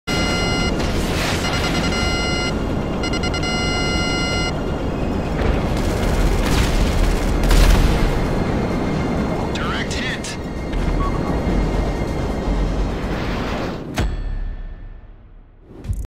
Direct HIT
Some funny coincidence happened while I'm doing the heist preparing mission.